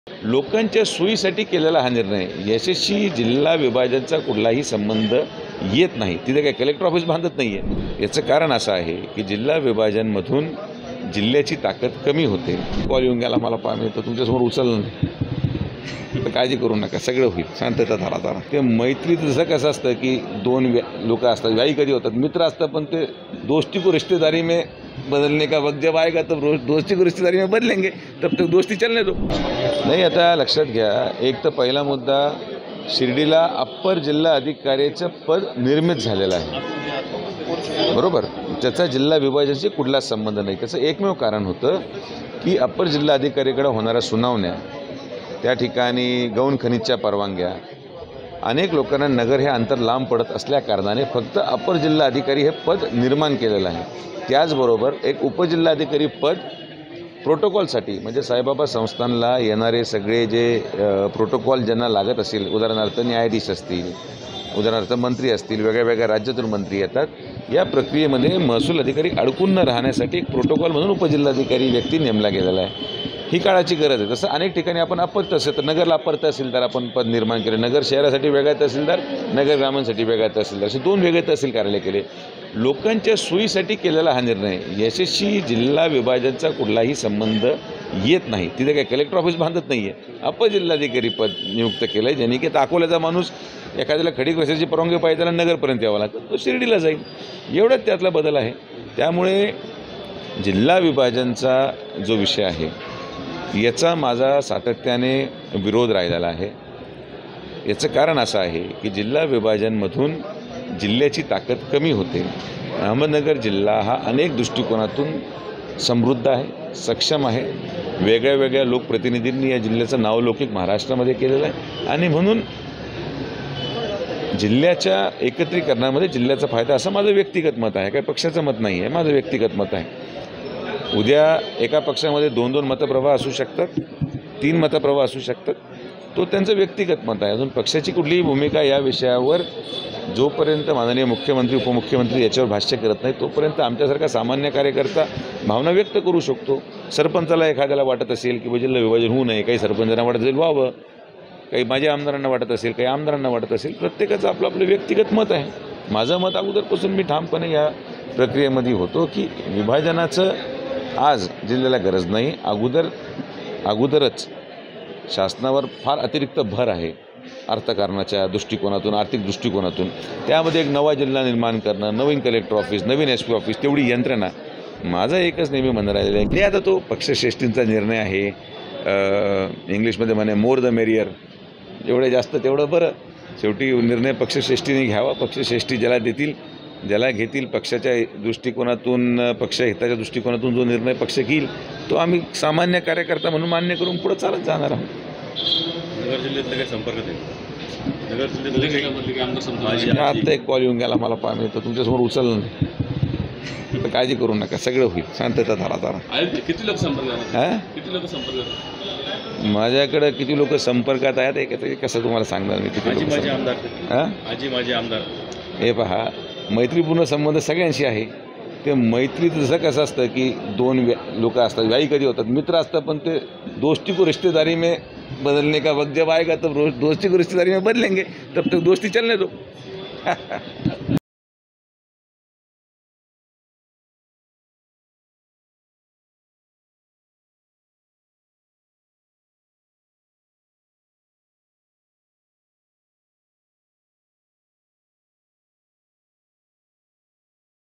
Sujay Vikhe : आत्ताच मला पारनेरमधून एक फोन येऊन गेला... | LetsUpp Marathi
लोकसभेच्या उमेदवारीबद्दल तसेच इतर पक्षातील आजी-माजी आमदार विविध कार्यक्रमाच्या माध्यमातून आपल्या संपर्कात आहेत का? यावर विचारले असता खासदार विखे म्हणाले, राजकारणात माझे अनेकांशी मैत्रीपूर्ण संबंध आहेत, वेळ आल्यावर यावर बोलता येईल.